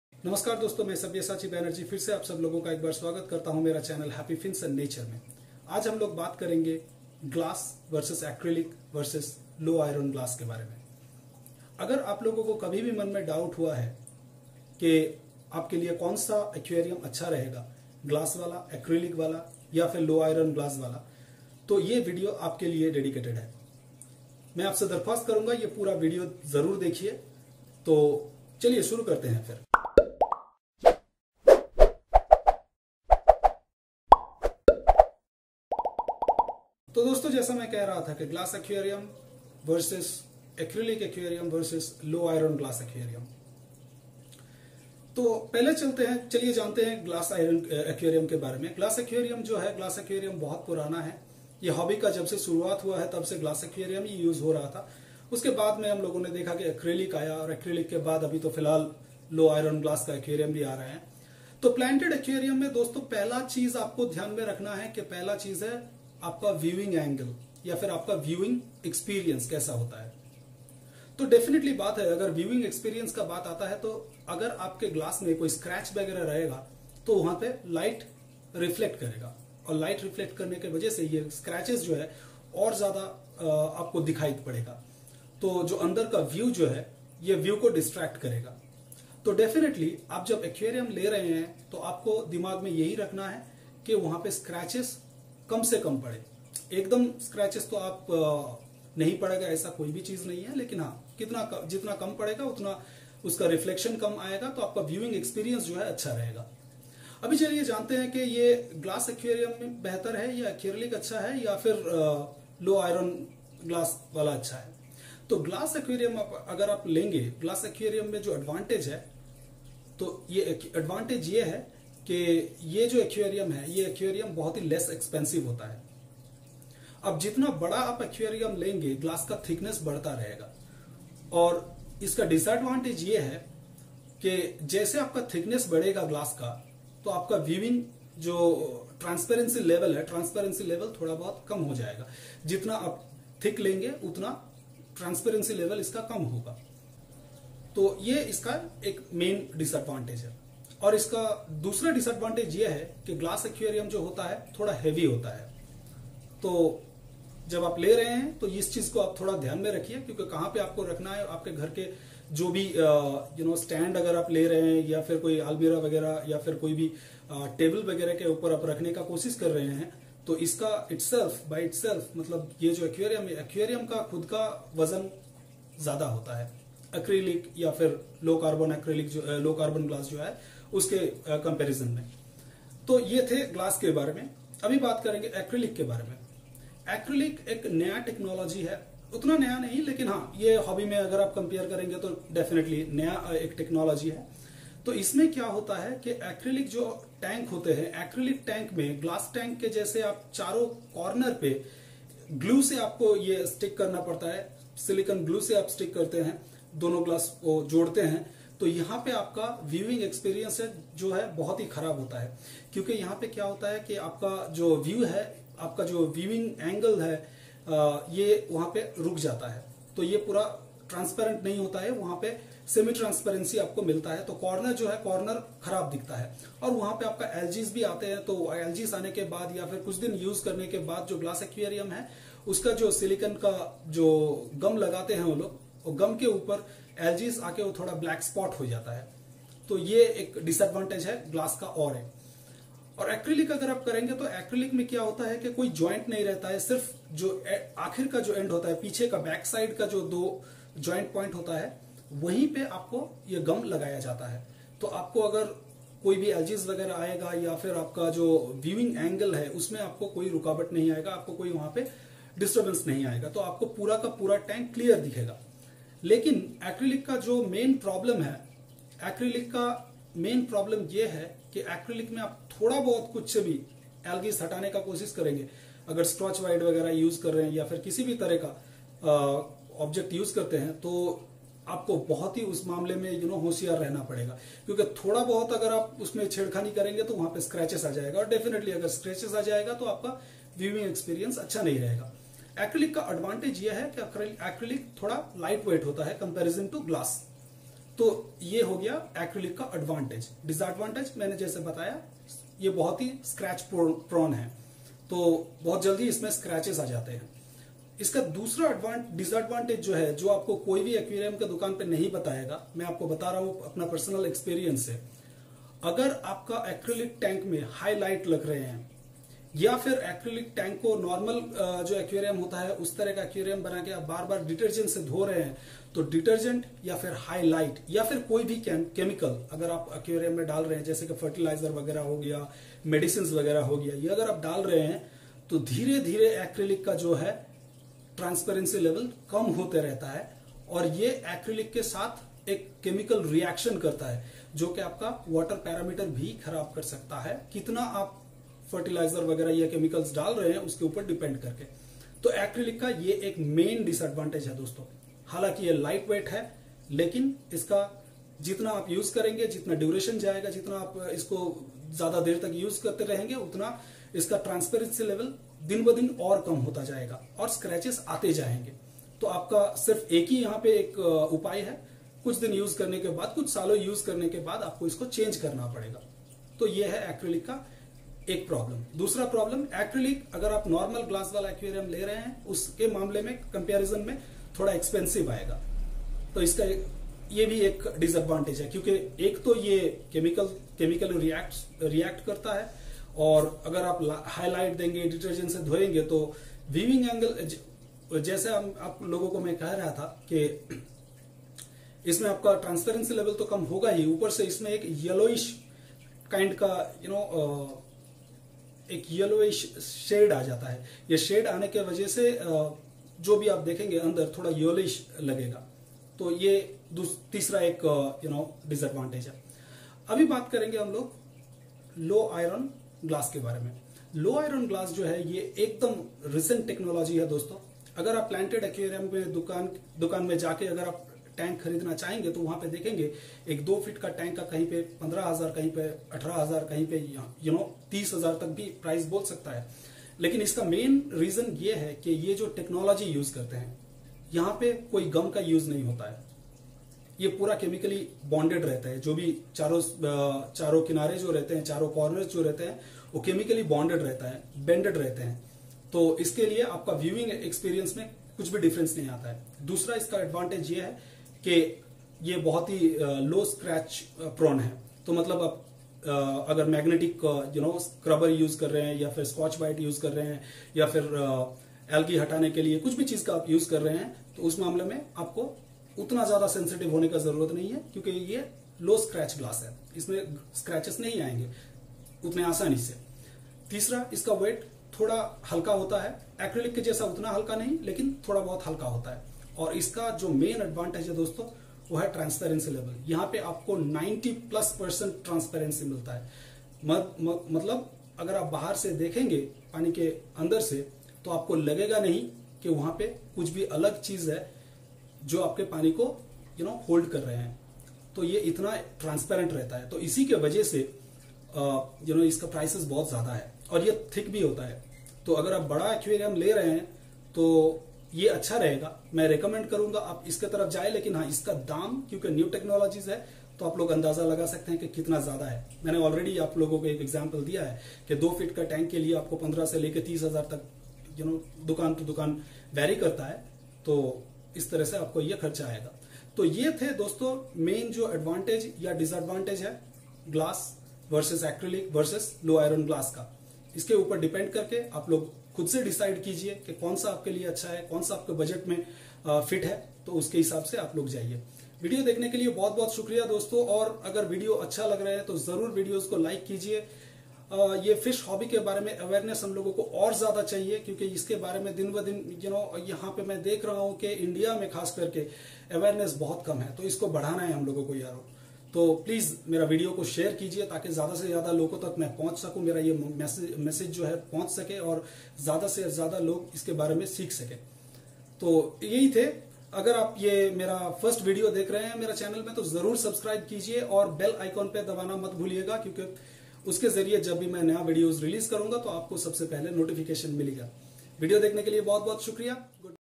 HOW TO CHOOSE: Glass Vs Acrylic Vs Low iron Glass aquarium in hindi.
Guys Here in this video I have explained how you can choose which type of material to choose from glass vs acrylic vs low iron glass.

Well it all seems the same but there are a lots of difference and lots of pros and cons:

Let's discuss the advantages and disadvantages of all.

Glass:
Advantages:
Glass is low cost.

Disadvantages:
Prone to scratches.
Less transparency.
Low viewing experience due to the corner joints.

Acrylic:
Advantages:
Light weight.
More transparency during the initial period of purchase.
Different shapes and sizes.

Disadvantages:
Prone to scratches,
Tough to clean.
Chemically reacts.
Gets yellowish with time due to high light and detergent.
Expensive.

Low iron glass:
Advantages:
High transparency.
Very durable.
Scratches proof.
Different sizes and shapes available.
Easy to clean.
Light weight.
Awesome viewing experience.
No corner joints.

Disadvantages:
Very expensive.

So there were the pros and cons.
I will keep it to you guys which one to choose. For a beginner I would prefer regular glass ones but it is completely up to you.

I hope this have been informative and it will help you to choose wisely as per your budget.

Wish you all the very best.

Happy fins and nature
Sabyasachi Banerjee

fishyfishy06 - fishbank